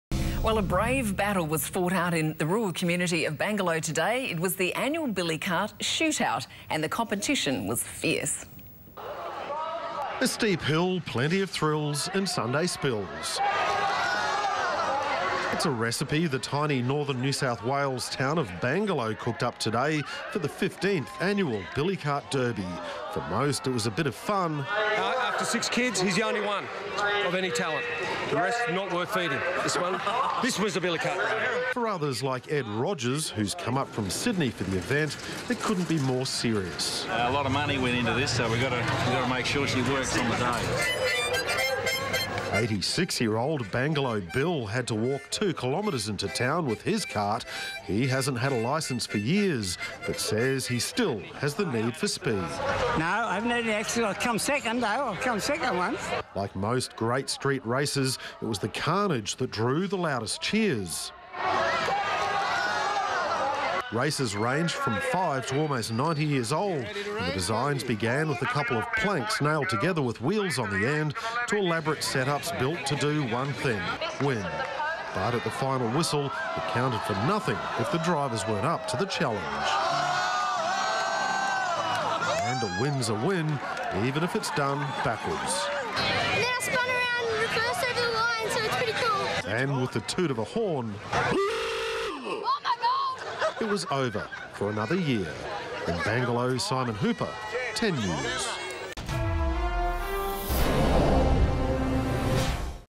Bangalow Billy Cart Derby
A brave battle was fought out in the rural community of Bangalow on Sunday - It was the annual Billy Cart shoot out, and the competition was fierce!